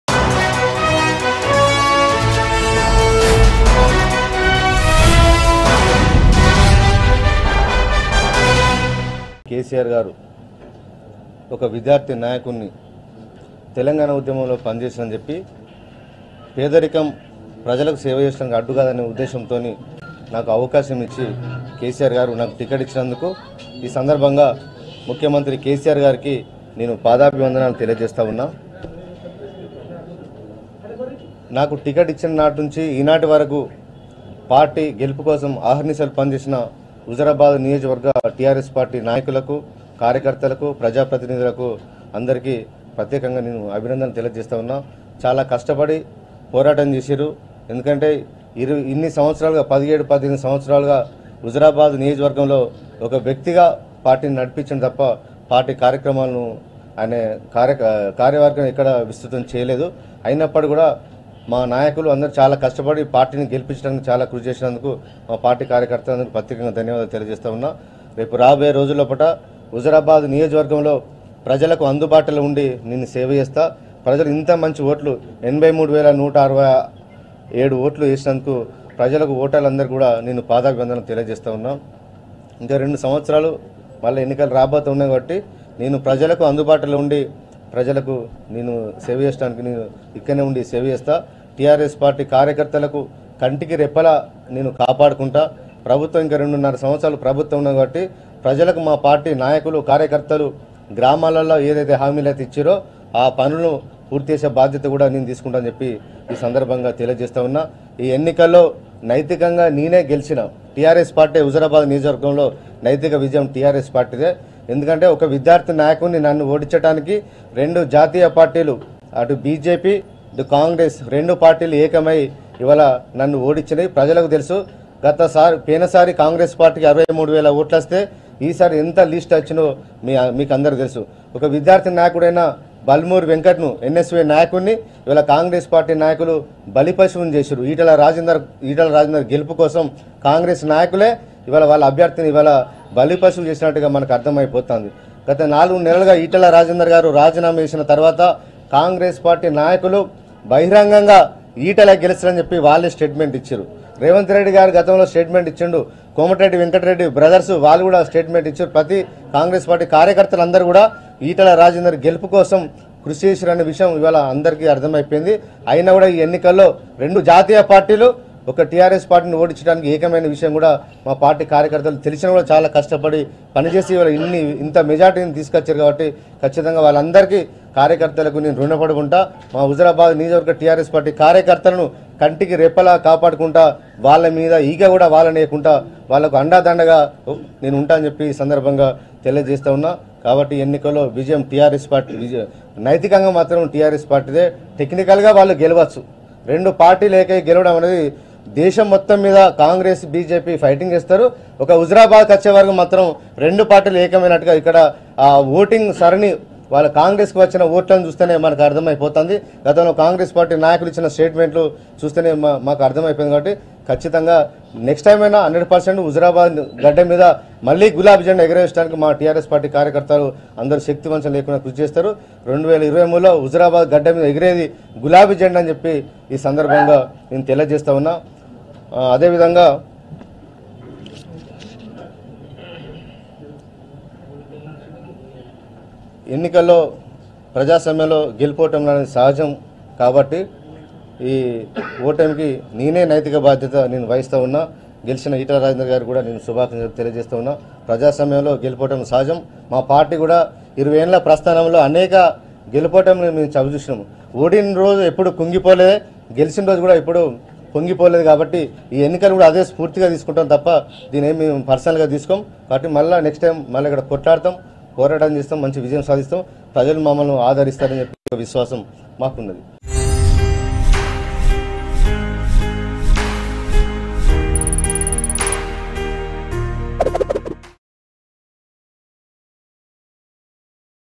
TRS Leader Gellu Srinivas Yadav Says His Heartful Congratulations To Etela Rajender | Mango News
TRS Leader Gellu Srinivas Yadav Says His Heartful Congratulations To Etela Rajender.